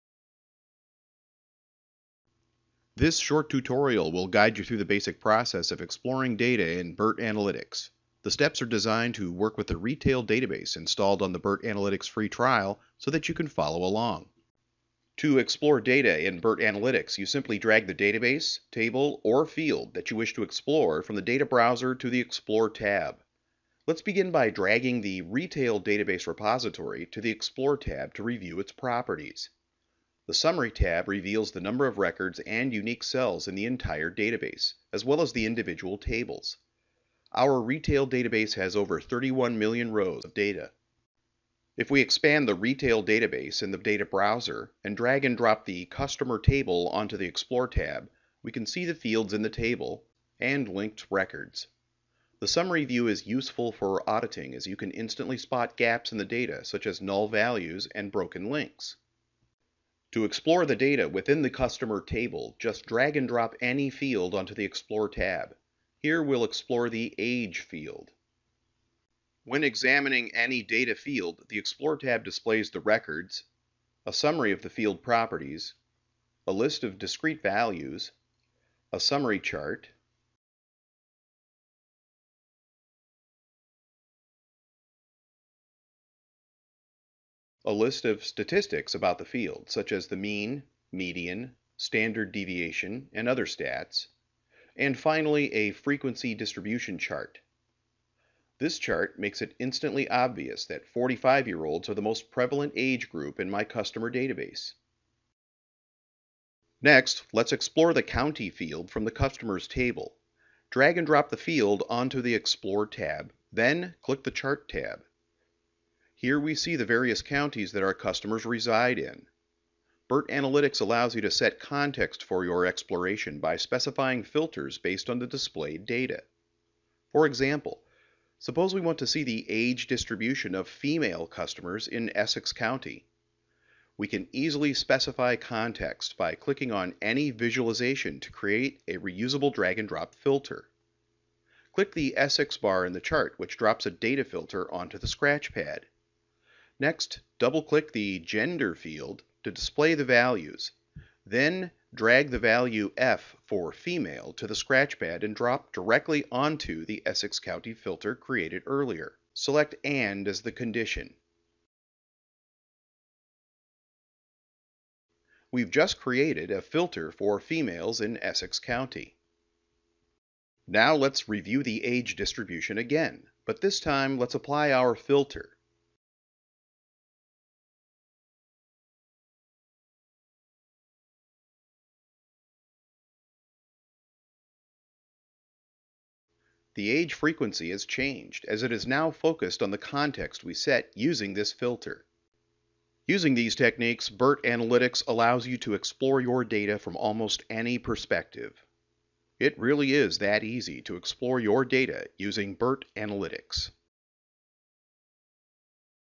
Visually Explore Big Data with BIRT Analytics!
BIRT Analytics makes it easy to explore your data, even data with millions of records. Uncover useful information instantly and focus on specific perspectives or context as you go. This tutorial will guide you through the basics of data exploration in the BIRT Analytics online trial application.